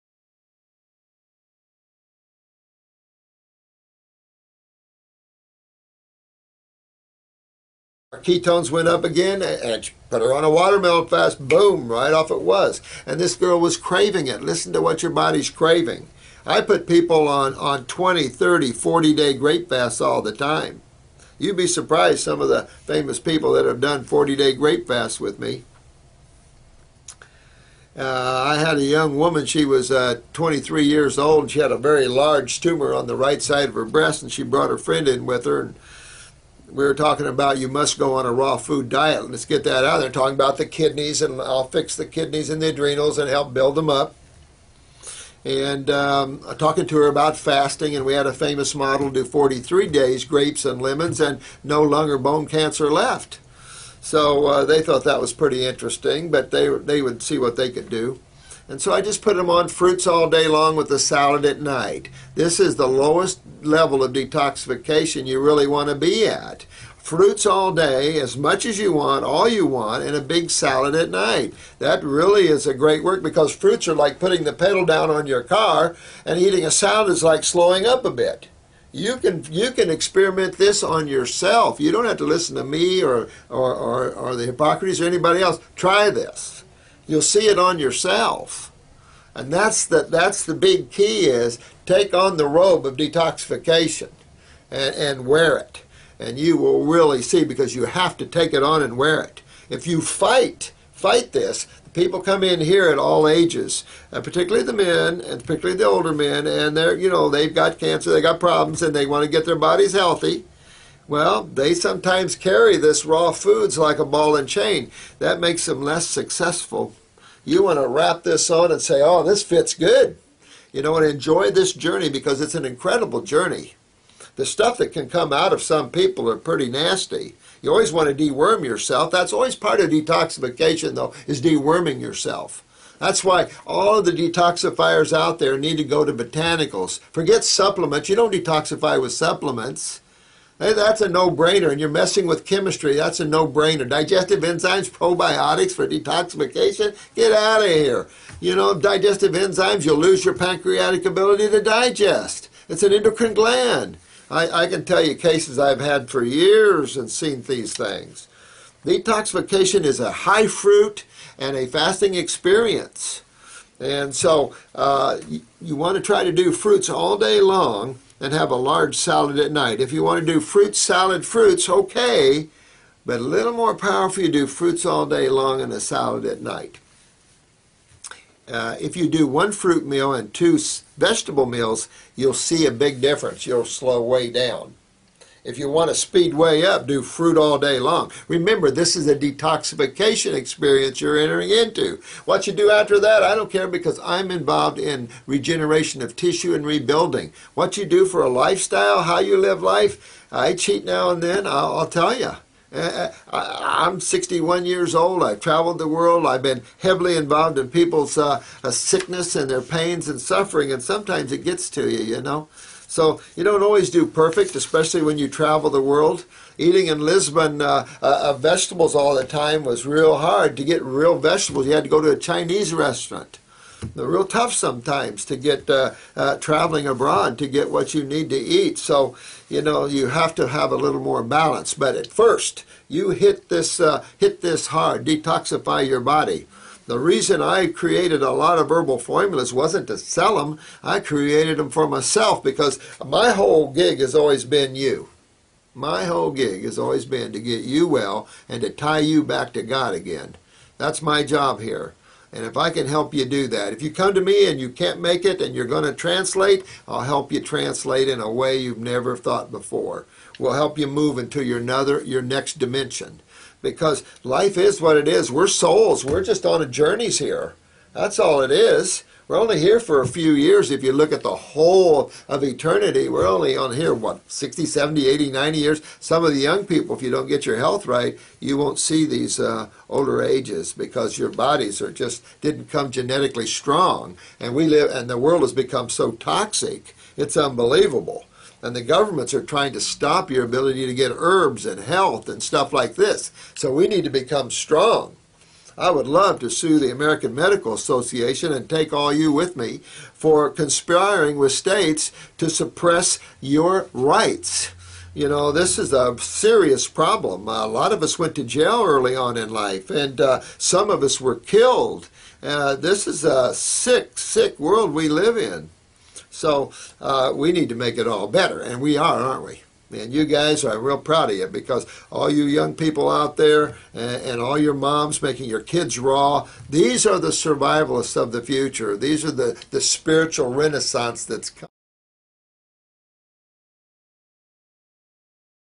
Detoxification The Art & Science Part 5
Dr. Morse speaks on the science and art of detoxification.